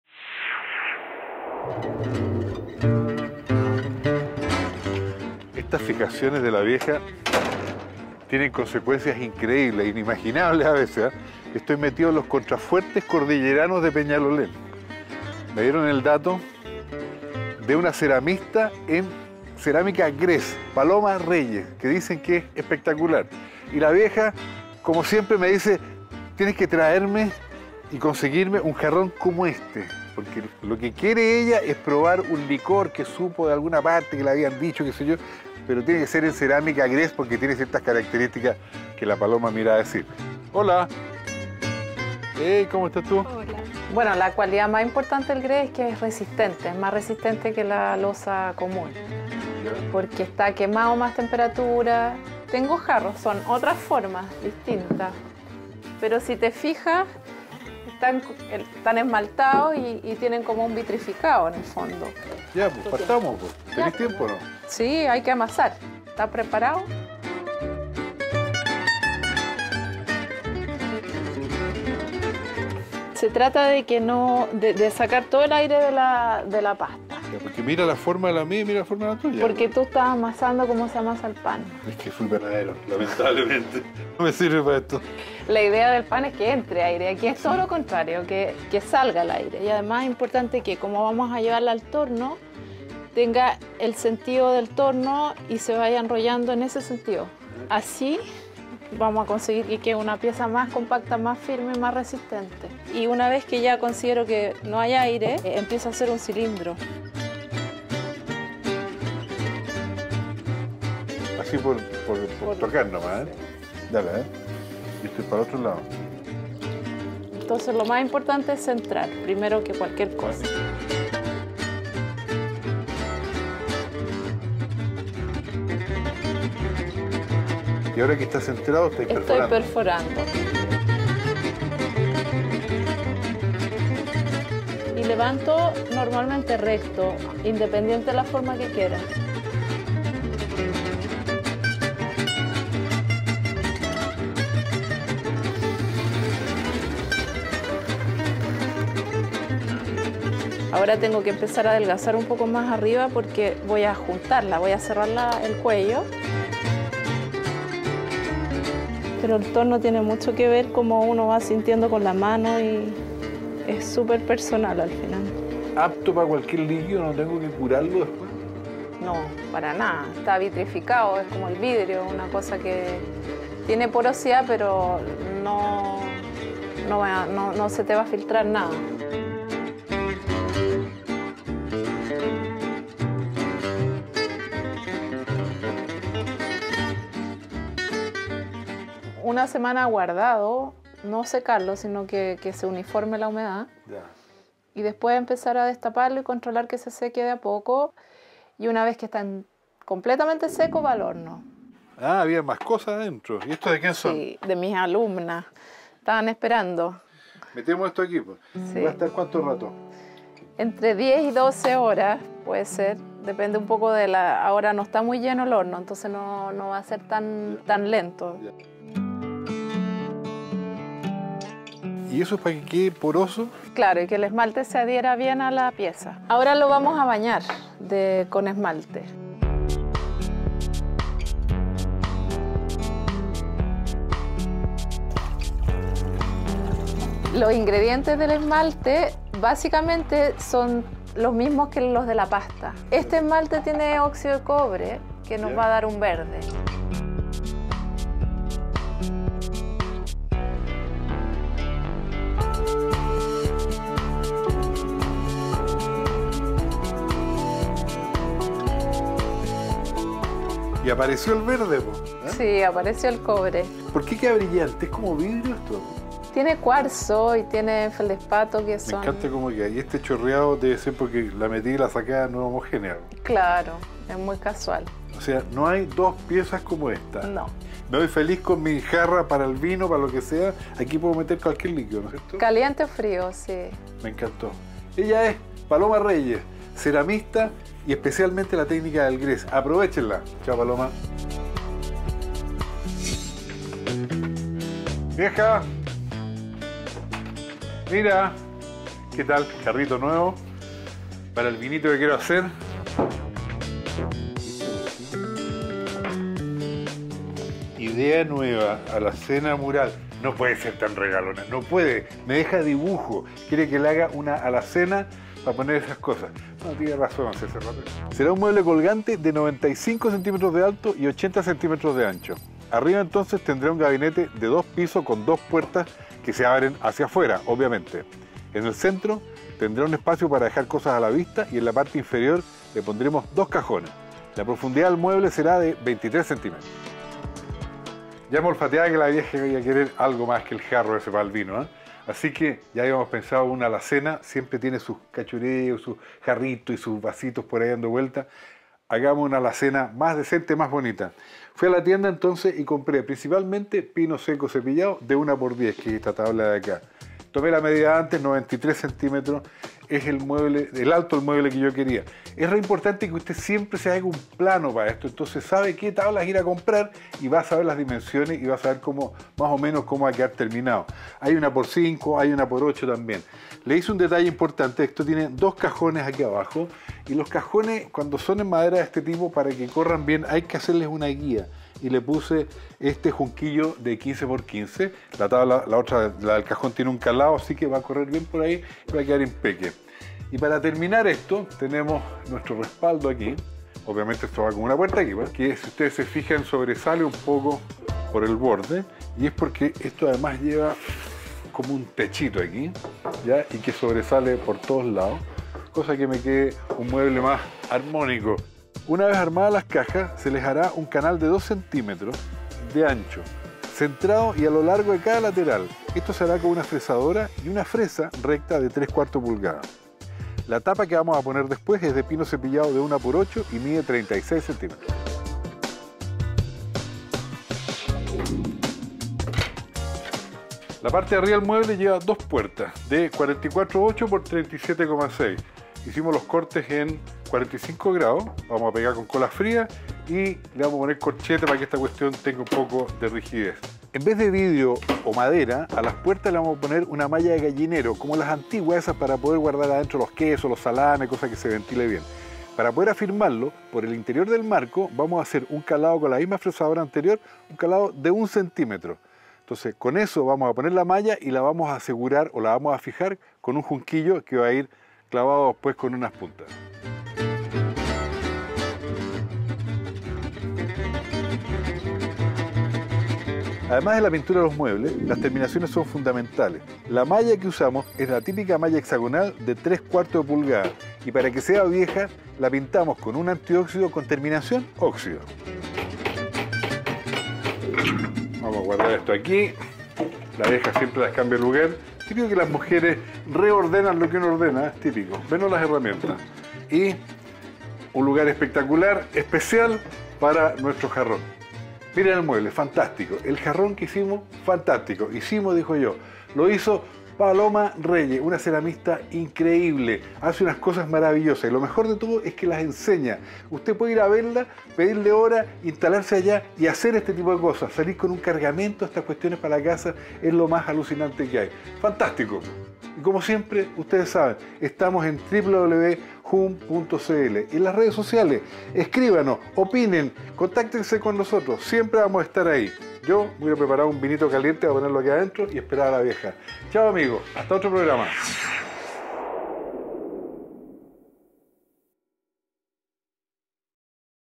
¿Cómo hacer una alacena rústica para la cocina?
Una alacena es un armario con puertas y estantes que se usa para guardar alimentos o poner el menaje de cocina. La haremos rústica para que contraste y resalte el trabajo en platos y jarros realizados, en esta ocasión por una ceramista.